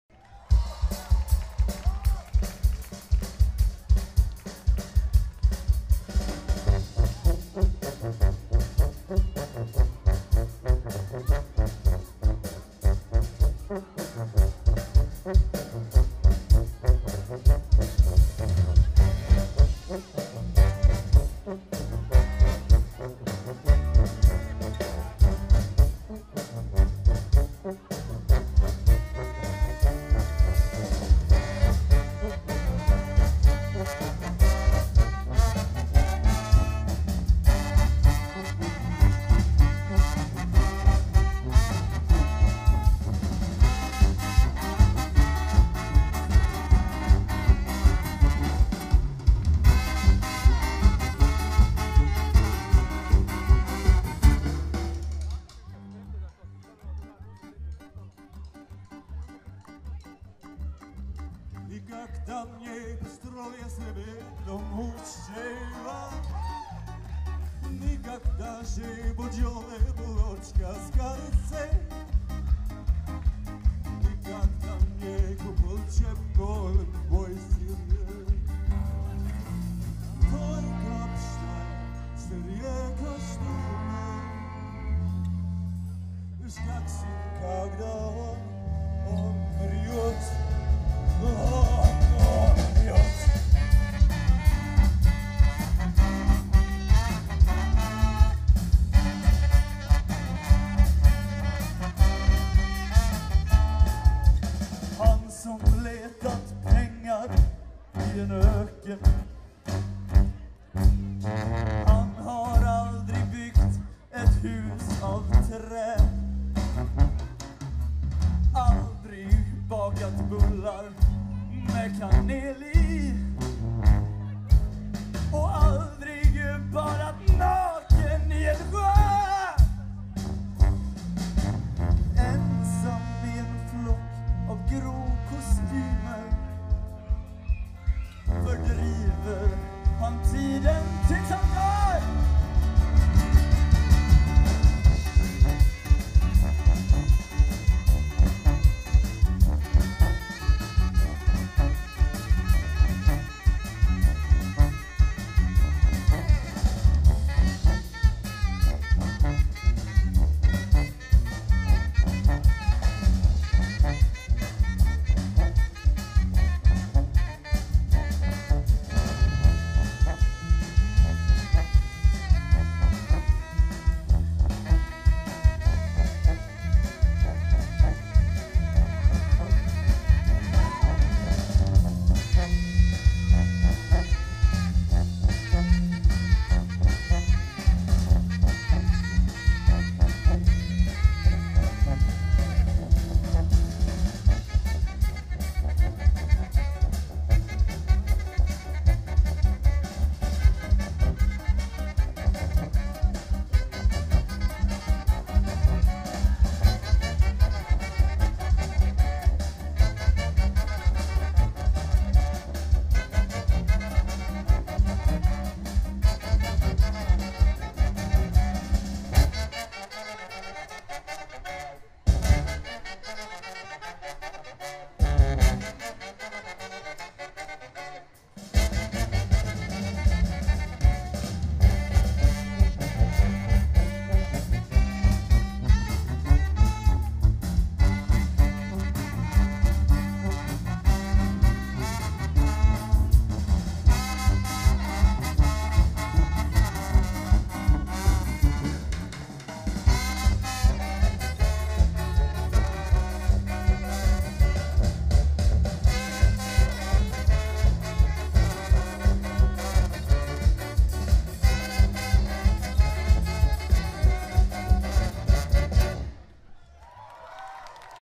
Halkan Balkan Live@picknickfestivalen 2012 (Klipp 2)
Halkan Balkan röjer loss i Svarte Mosse på Hisingen.

Inspelat den 6 juni 2012.